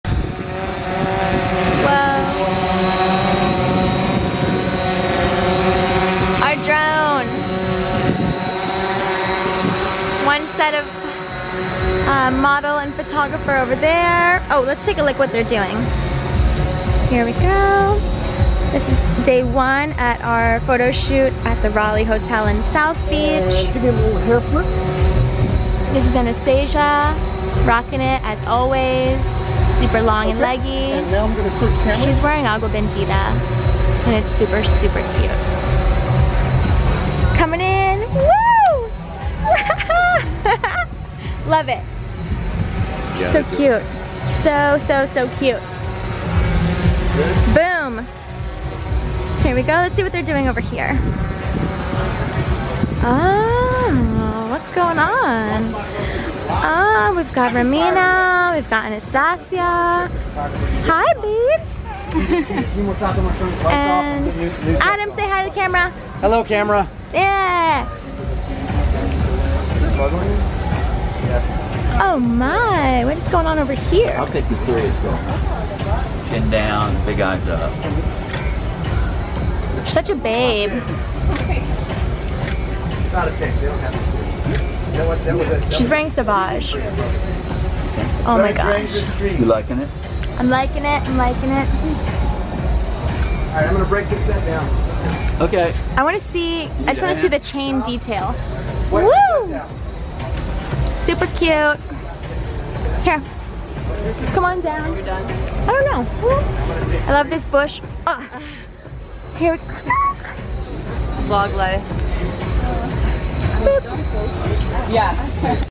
Behind the Scenes with Bikini Luxe
We spent three days at a beach front hotel, had four models, six photographers, and fifty designer bikinis. What's not to love? The Raleigh Hotel in South Beach provided the perfect backdrop for our swimwear photo shoot, and we got to shoot all throughout the hotel, but the pool was our favorite.

We teamed up with Leica Camera to ensure that the photo quality was on point and print quality. The models wore luxury swimsuits by brands featured such as Frankie's Bikinis, Agua Bendita, Luli Fama, Dbrie Swim, Sauvage Swimwear and more. Here's a sneak peek from one of our Instagram Live videos of what the process looked like. Hope you enjoy!